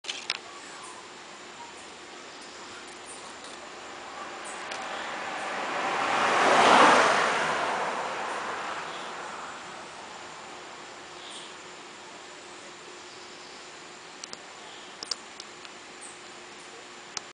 Iguane à Deshaies
Un jeune iguane coincé dans une grille.